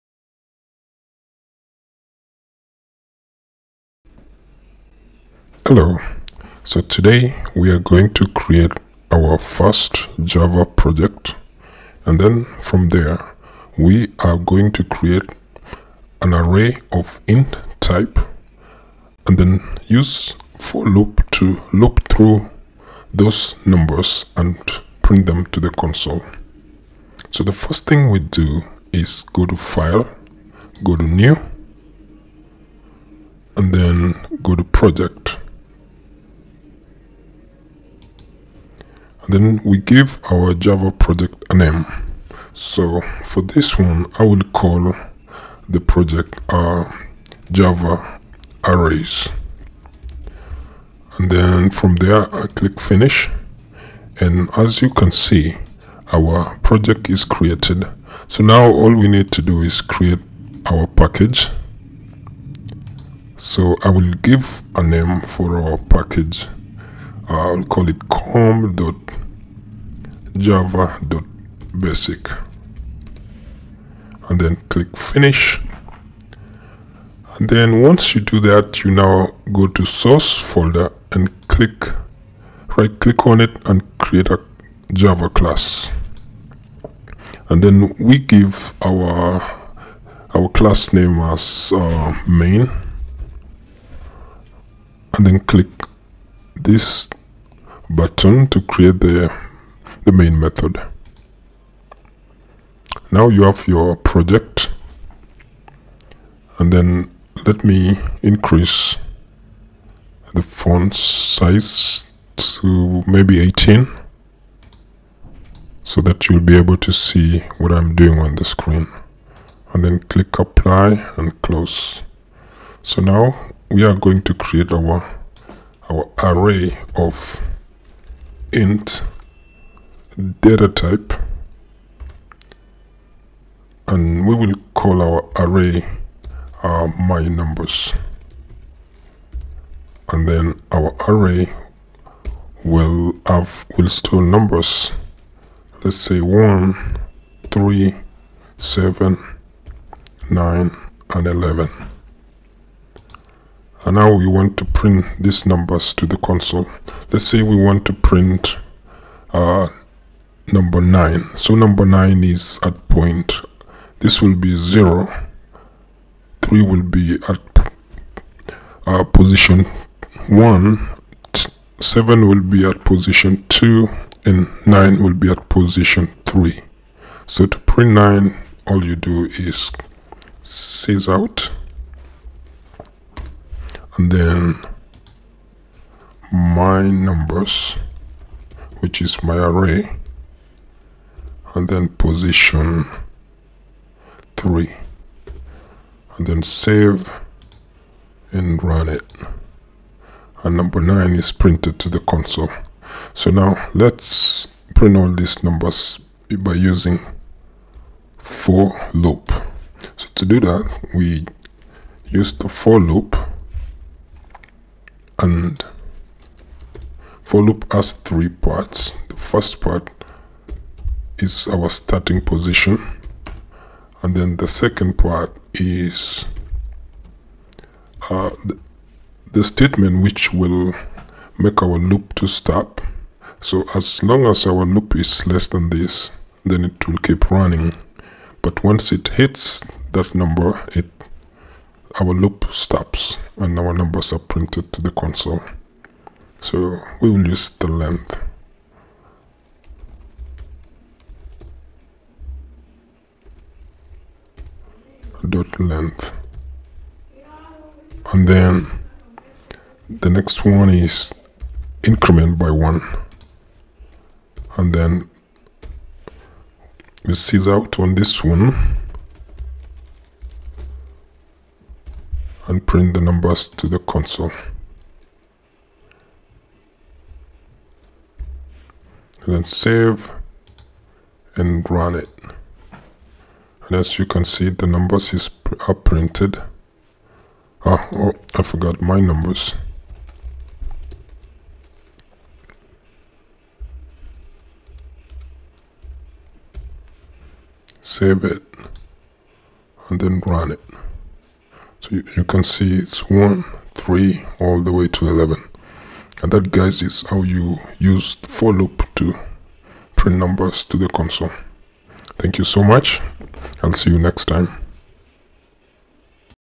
Java Arrays | For Loop in Java
How to loop through an array in Java and print values to the console.